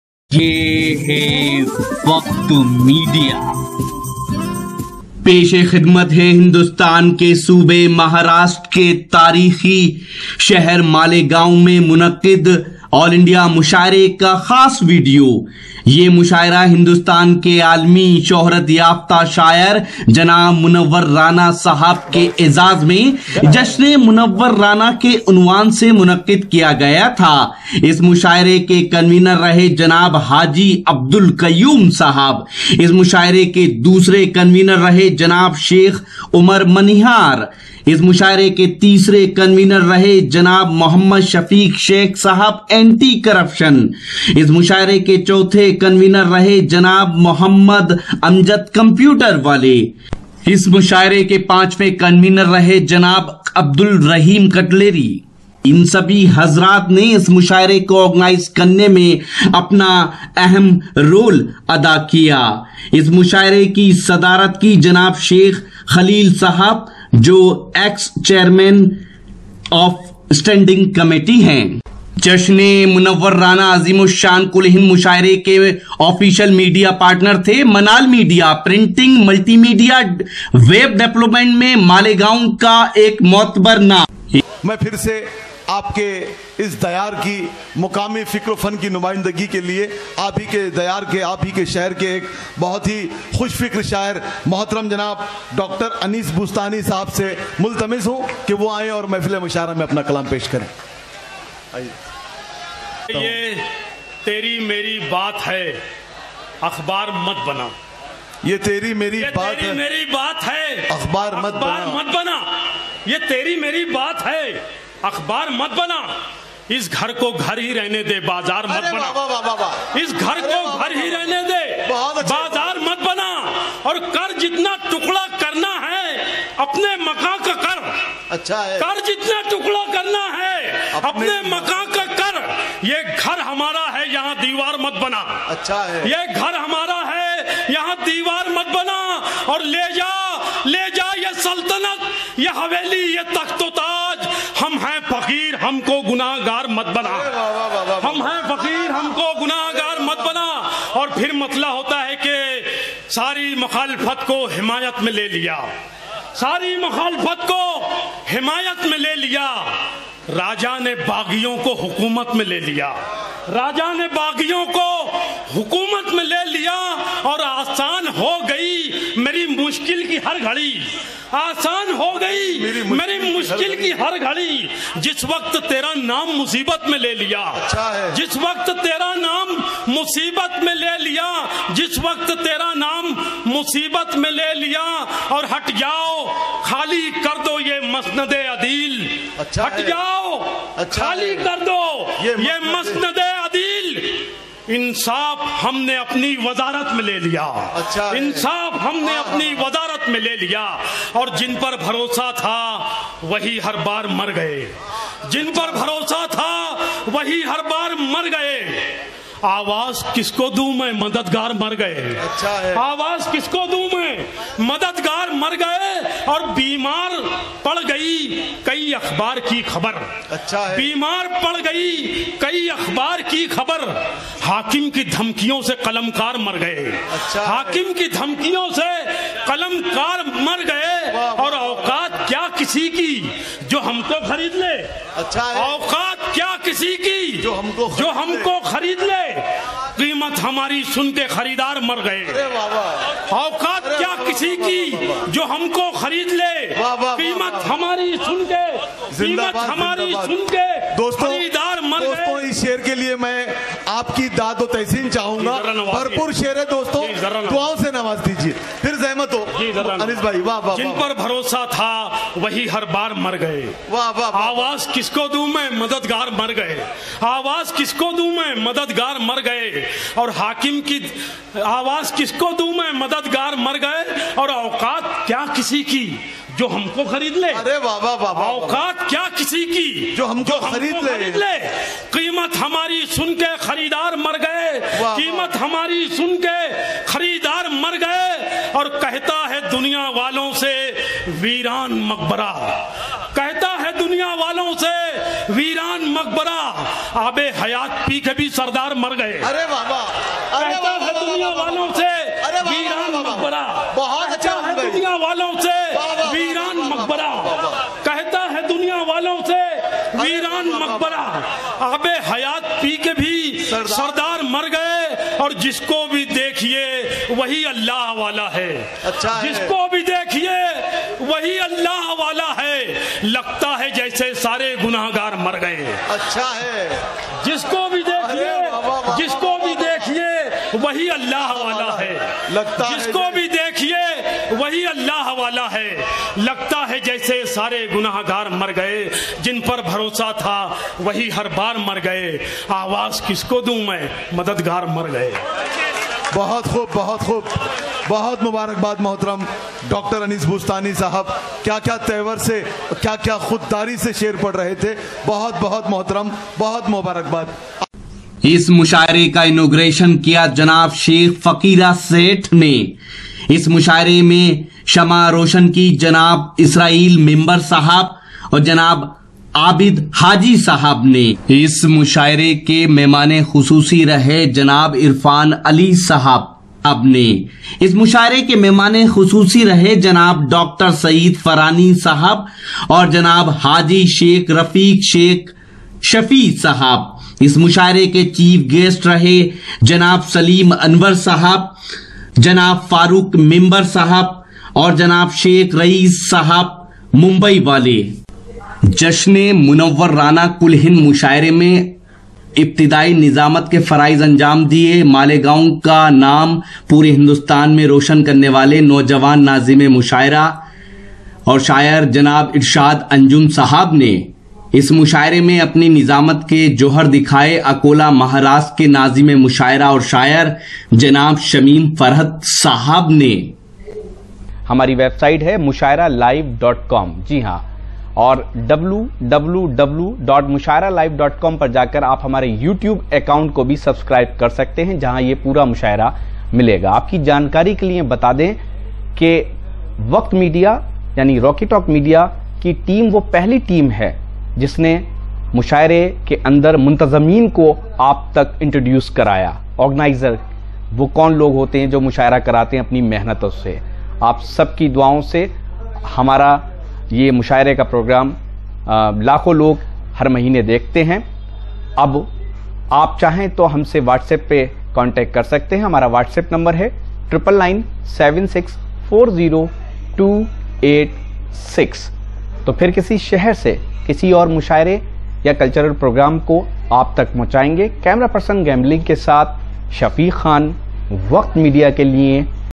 Anis Bostani Malegaon Mushaira 2016 Jahsn e Munawwar Rana 2016
Jahsn e Munwwar Rana All India  mushaira     was  Held  on  9 July   2016 covered by Waqt Media ( in association with RockeTalkMedia  Team ) from  Malegaon  Maharashtra   India
Convener-
Haji Abdul Qayyum
Abdul Raheem Katleri
Sheikh umar manihar
Mohd amjad computer
Presiding Guest- Sheikh Khaleel Sheikh Shafi
Mushaira Inauguration – Fakeera Seth
Shama Farozi- Israil Member & Abid Haji
Chief Guests- Dr Saeed Farani,  Haji Shaikh Rafique Shaikh Shafi
Saleem Anwar, Shaikh Raees Mumbai, ) Farooqu Member
Nizamat- Shameem Farhat
Poets-
Munwwar rana, Dr Naseem Nikhat, Aadil Rasheed, Chandni Shabnam, Wahid Ansari, Mushtaq Ahmad Mushtaq, Irshad Anjum, Saif Babar, Shahid Aanjum, Javed Allahabadi & Many More